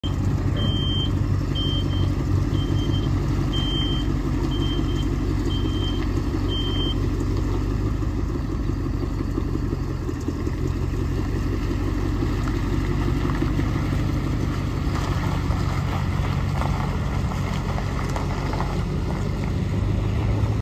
2011 Ford F350 Crew Cab Utility Truck
Location: Washougal WA ***End-User Consignment*** VIN: 1FD8W3HT1BEB34261 Engine: Powerstroke 6.7L Diesel Transmission: Automatic *Possible trailer brake issue*, Knapheide utility body w/6 compartments, running boards, tow package, dual fuel tanks, power windows, power mirrors, power locks, cruise control, A/C, AM/FM, LT245/75R17 tires. Miles: 243,752

Year: 2011
Make: Ford
Miles: 243752
Model: F350